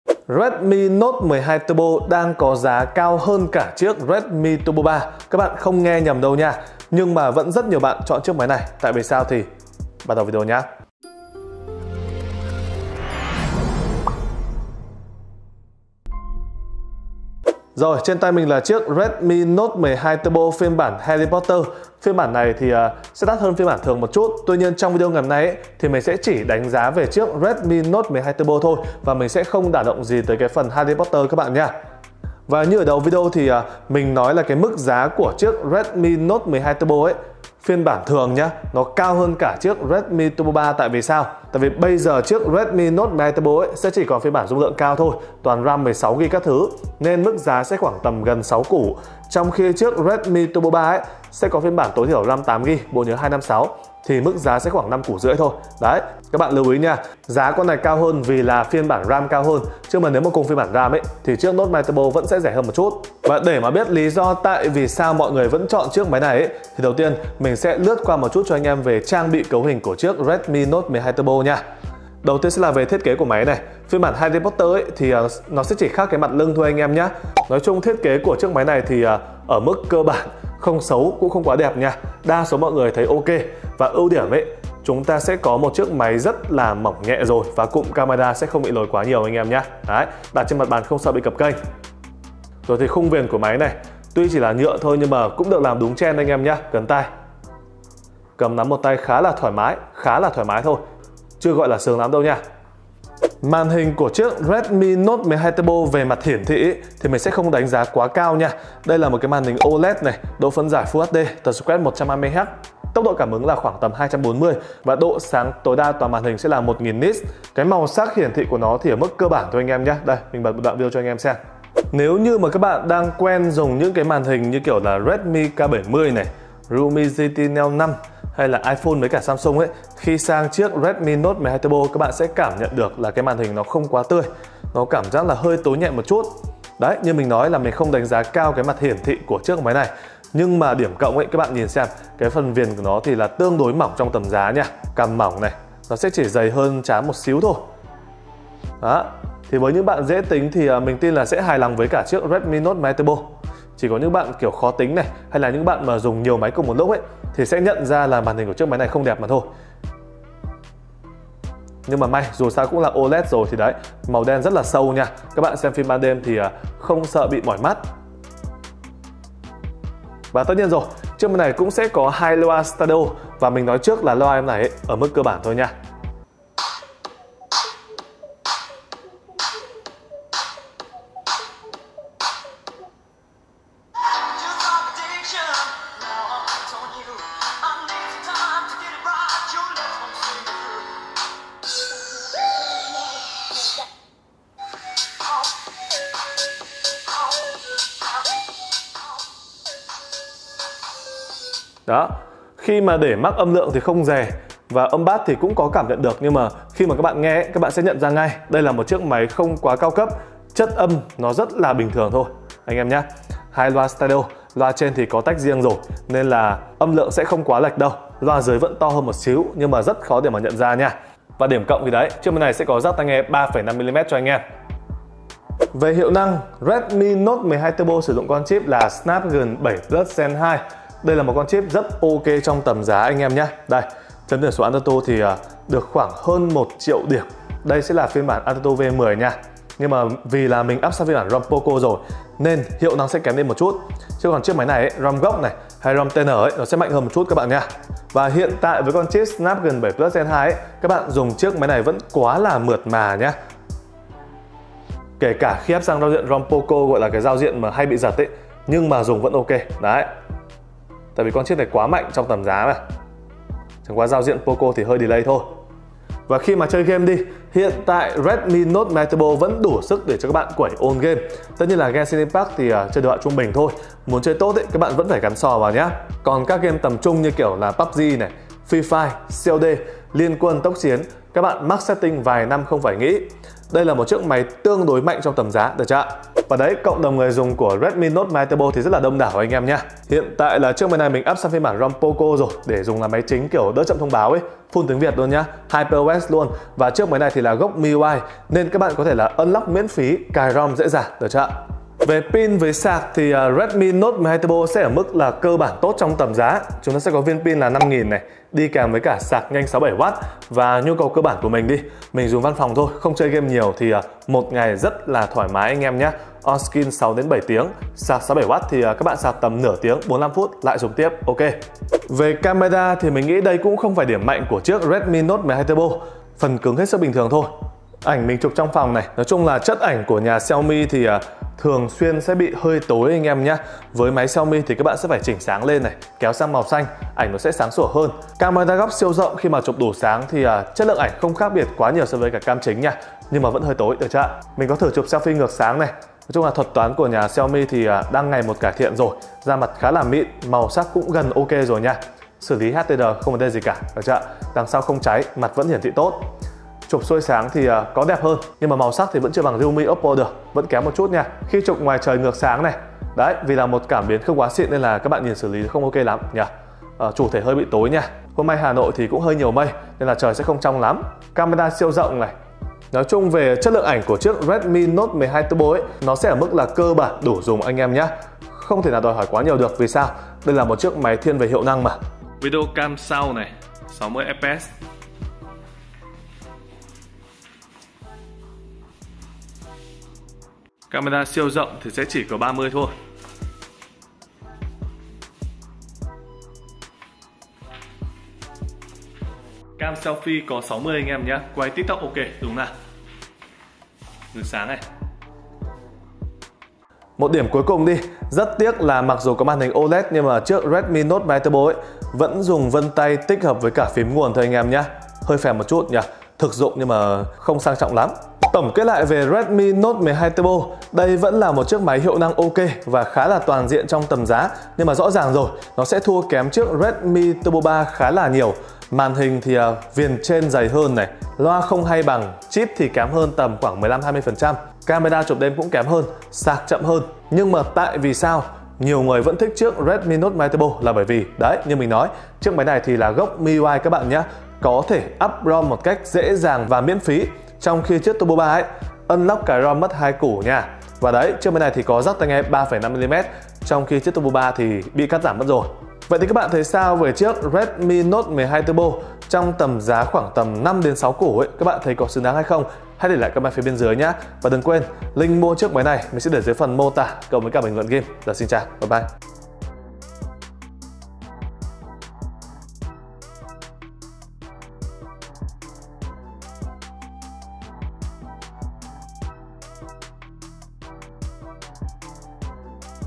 Redmi Note 12 Turbo đắt hơn cả Redmi Turbo 3 nhưng vẫn HOT? Lý do là vì…
Nội dung:
0:00 Đánh Giá Lại Chi Tiết Redmi Note 12 Turbo
0:17 Mức Giá Redmi Note 12 Turbo
1:05 Thiết Kế Redmi Note 12 Turbo
1:43 Màn Hình Redmi Note 12 Turbo
2:55 Loa Redmi Note 12 Turbo
3:45 Hiệu Năng Redmi Note 12 Turbo
5:01 Pin Redmi Note 12 Turbo
5:20 Camera Redmi Note 12 Turbo
6:47 Vân Tay Redmi Note 12 Turbo